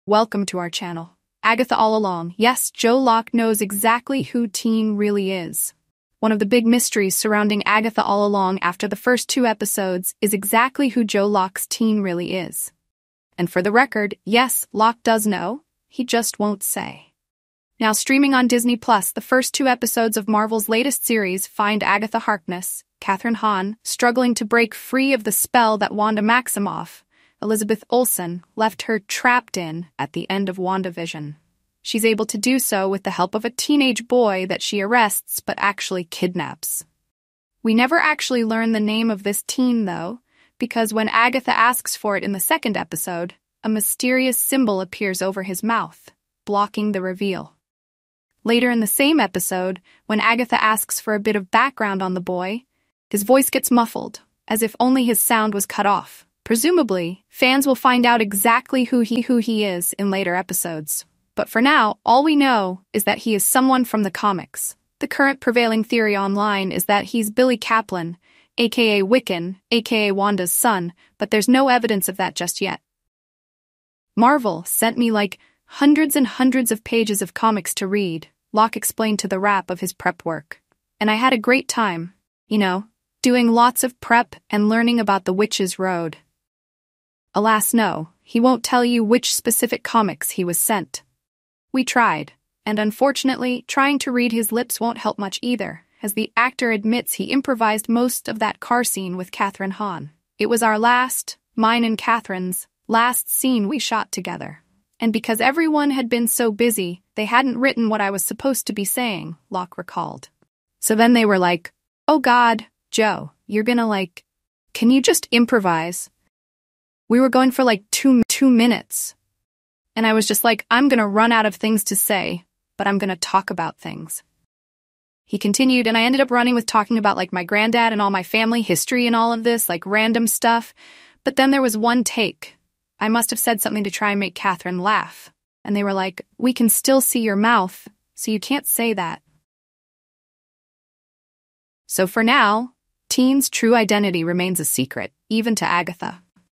Agatha All Along' Yes, Joe Locke Knows Exactly Who 'Teen' Really Is
#"Agatha All Along" creator Joe Locke has revealed that he knows the identity of his character, "Teen," but he won't reveal it. The first two episodes of the Marvel series, which premiered on Disney+, feature Agatha Harkness (Kathryn Hahn) trying to break free from a spell left by Wanda Maximoff (Elizabeth Olsen). She uses a teenage boy she arrests but kidnaps. However, the name of the teen is never revealed due to a mysterious symbol blocking the reveal. Locke admits to improvising most of the car scene with Kathryn Hahn, but the show's true identity remains a mystery. The prevailing theory is that he is Billy Kaplan, a.k.a. Wiccan, a.k.a Wanda's son, but there's no evidence of that yet.